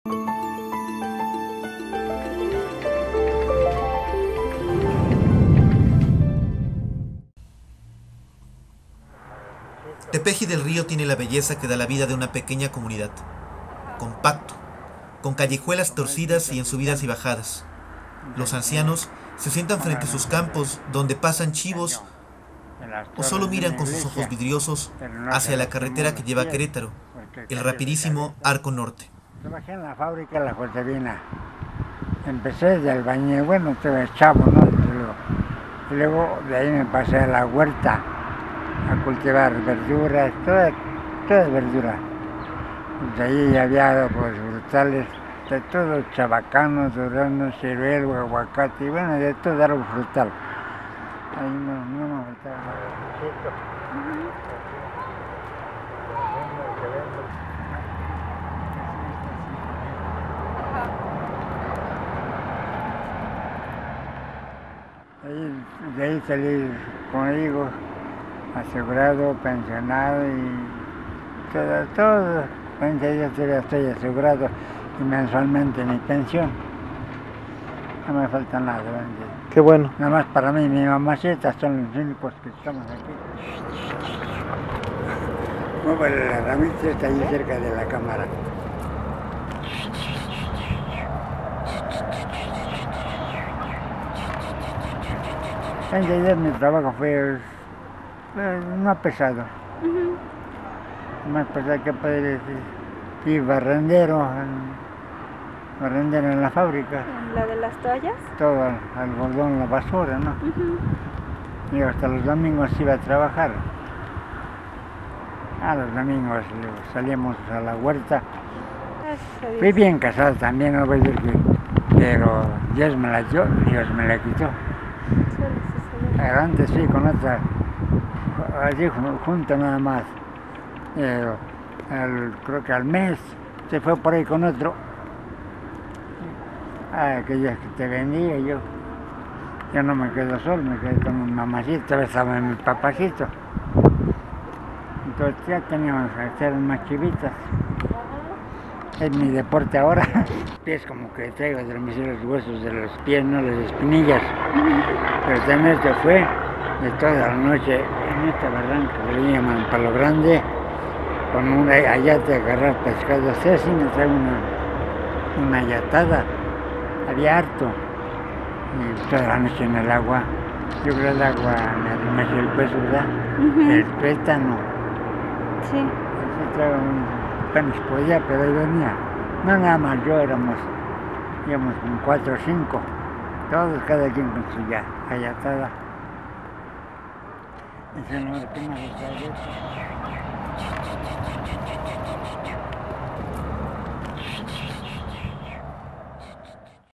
Testimonio en Tepeji del Río, Hidalgo
Tepeji del Río tiene la belleza que da la vida de una pequeña comunidad. Compacto, con callejuelas torcidas y en subidas y bajadas. Los ancianos se sientan frente a sus campos donde pastan chivos o solo miran con sus ojos vidriosos hacia la carretera que lleva a Querétaro, el rapidísimo Arco Norte.

Alfonso Santa Marín de 71 años ha vivido en esta comunidad toda su vida. En la fábrica La Josefina laboró como albañil, barrendero y velador. También se dedicó al campo, a la huerta y ahora, en su vejez, pastorea los chivos de su familia. Su madre aún vive y el anciano trabaja para cuidarla, para darle qué comer a "su jefecita".